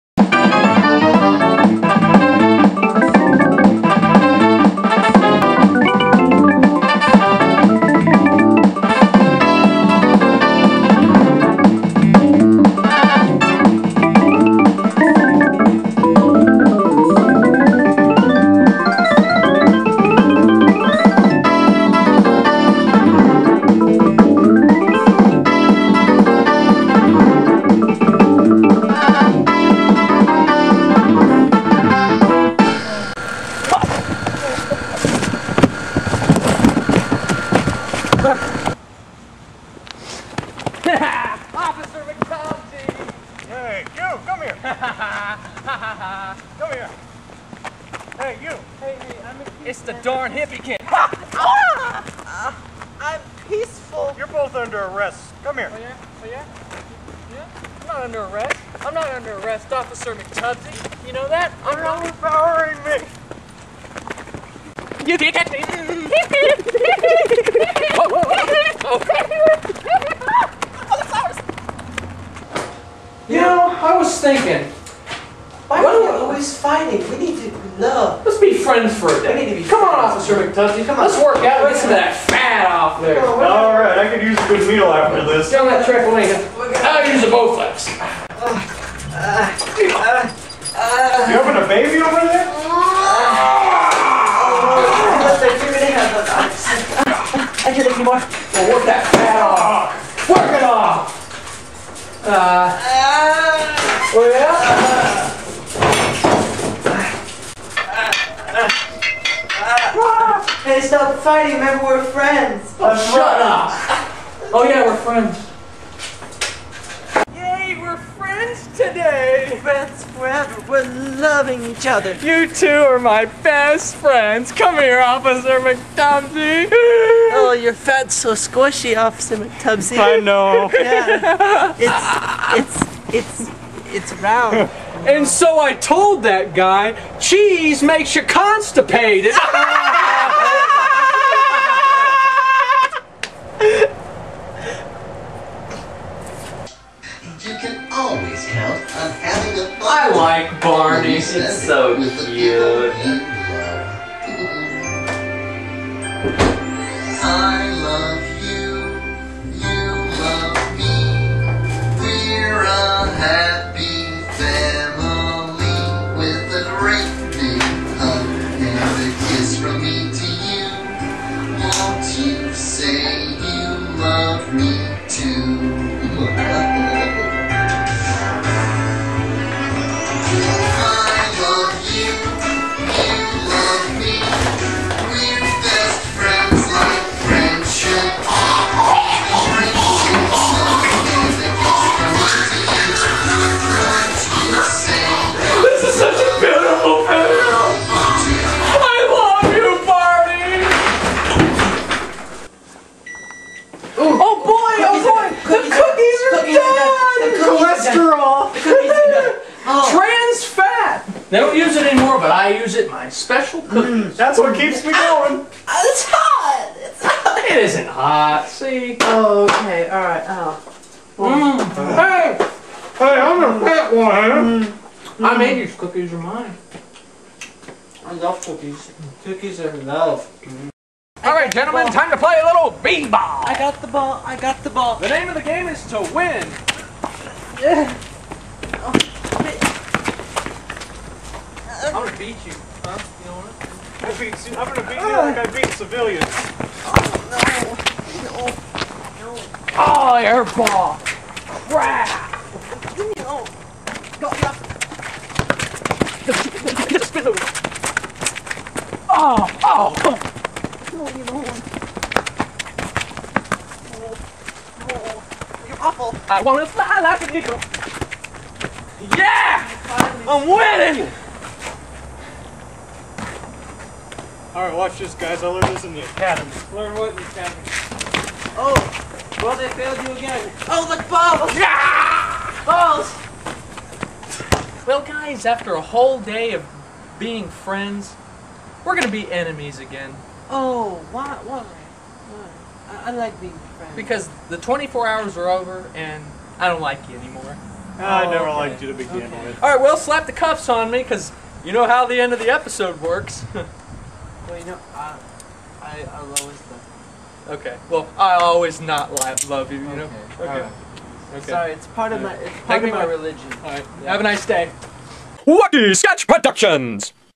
Unfortunate Hobo - Episode 14 "Friends for a Day"
The Hobo, Officer McTubsy, and the creepy Hippie kid decide that they should be Friends for a Day.